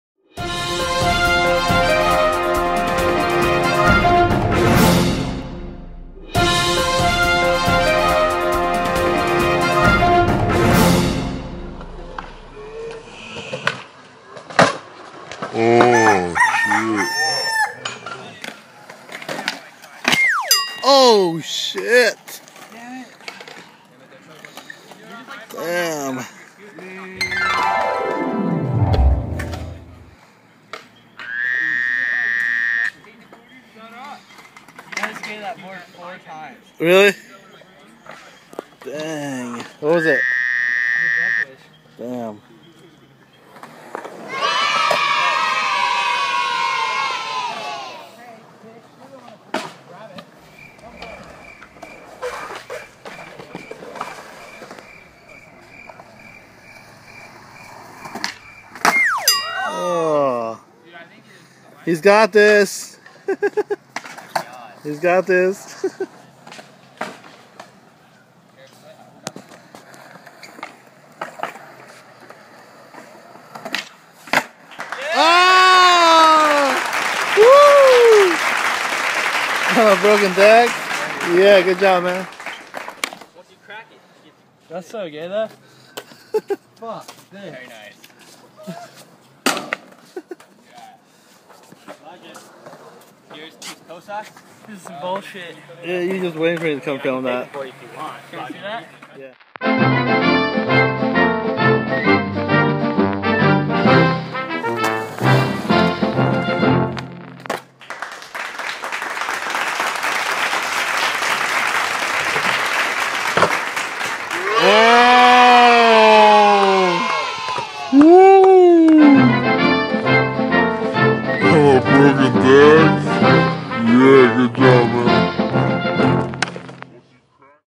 Grant Martin Trick of the Day #2!
Check out Grant Martin's Trick of the Day number 2!  Backside double big flip!!